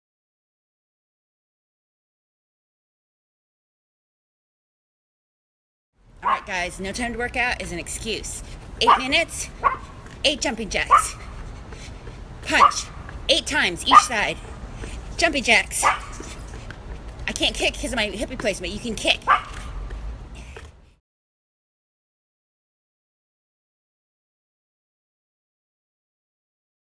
8 Minute Workout - No More Excuses!
Stop being lazy and coming up with excuses why you can't workout. It's FUN and necessary to your happiness, health, and sanity.

Start your day off right with just 8 minutes a day. You can do this!